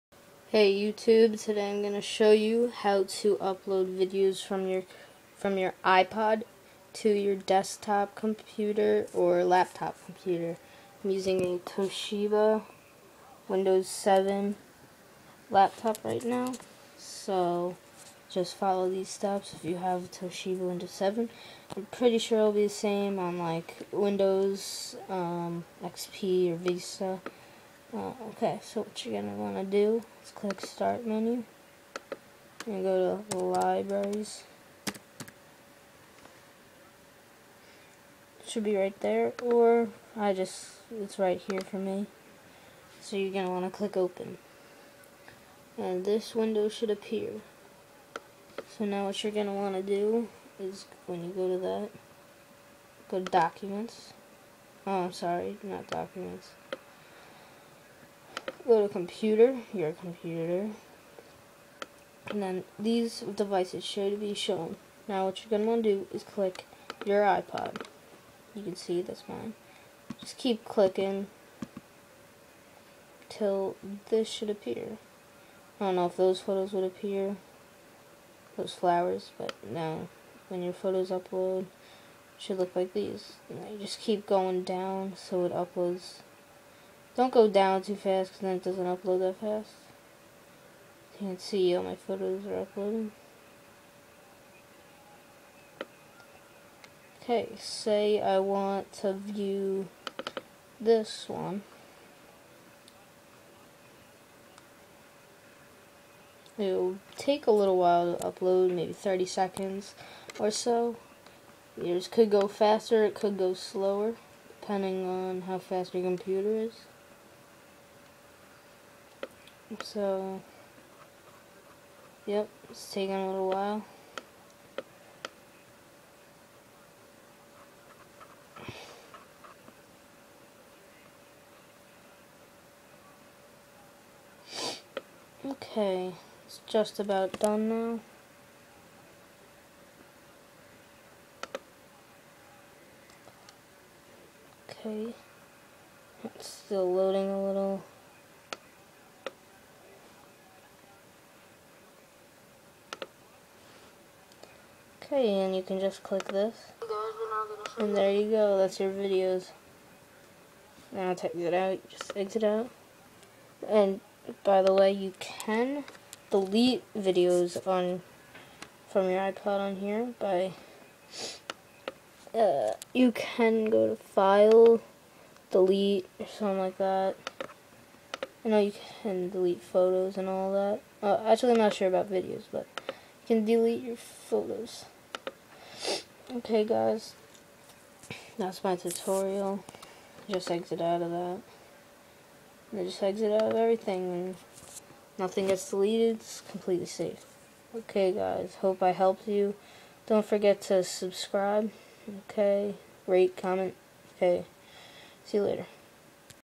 how to upload ipod pictures onto your computer
There has been a lot of problems with this so i thought that i could help you guys out. This also works for an ipad,iphone, or ipod 3g,2g,and1g, yes 4g. If you guys have any questions comment or send me a msg.